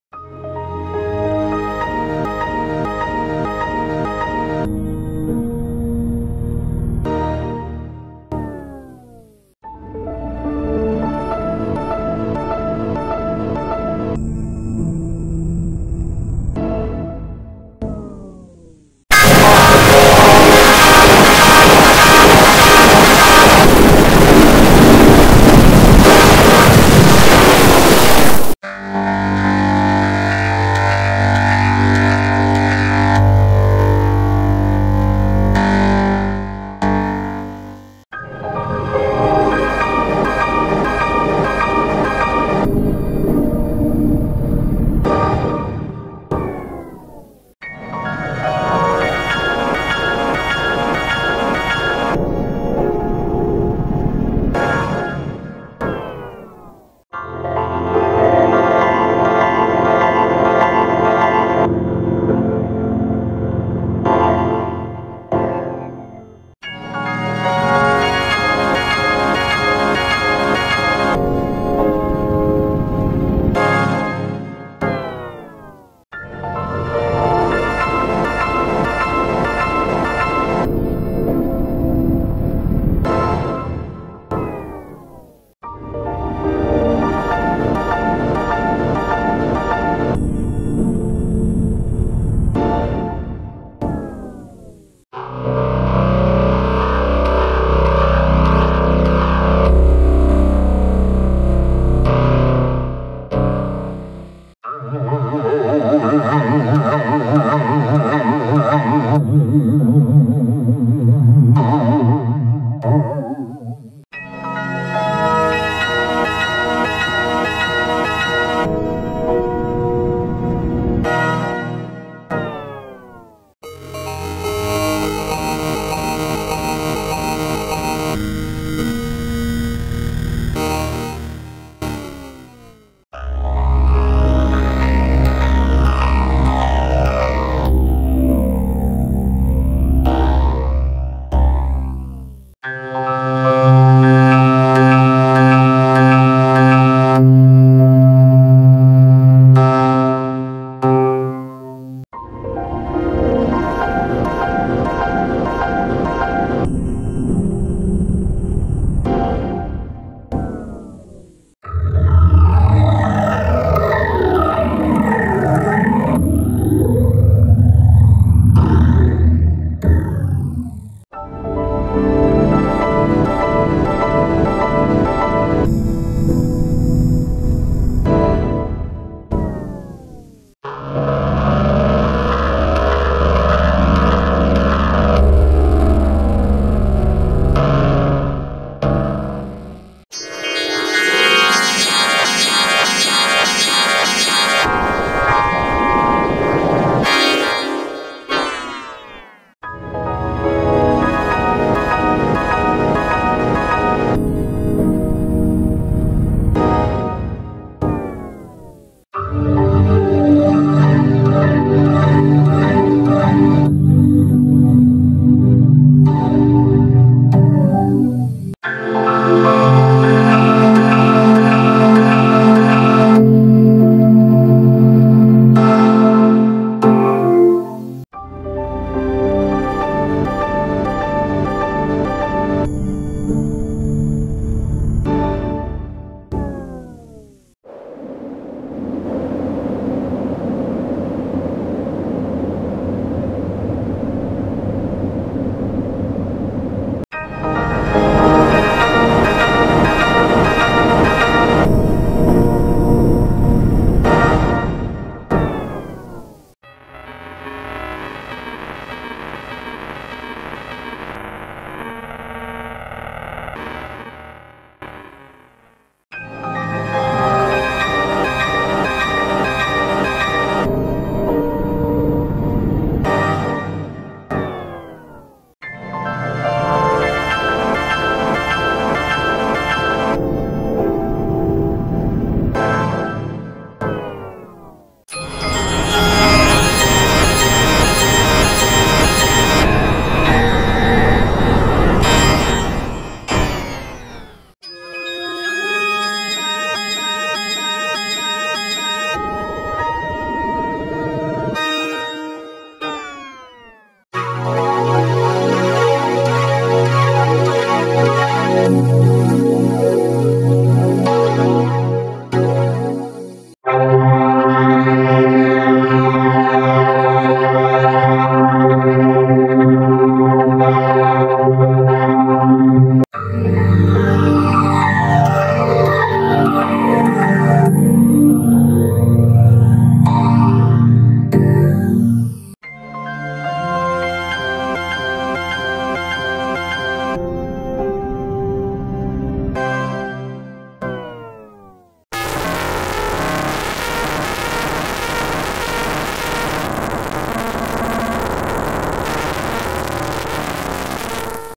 Klaskyklaskyklaskyklasky (Windows XP Edited Version) Effects 6 (My Fifth Preview)
Made on ‎Friday, ‎September ‎13, ‎2019, ‏‎10:15:58 pm to 10:24:38 pm

Here is a compilation of Klaskyklaskyklaskyklasky (Windows XP Edited Version) im my fifth preview. Warning!: The third and the thirty-seventh effects will get louder as scare!

A list of Sony Vegas Pro effects with links to tutorial video:

1. Neutral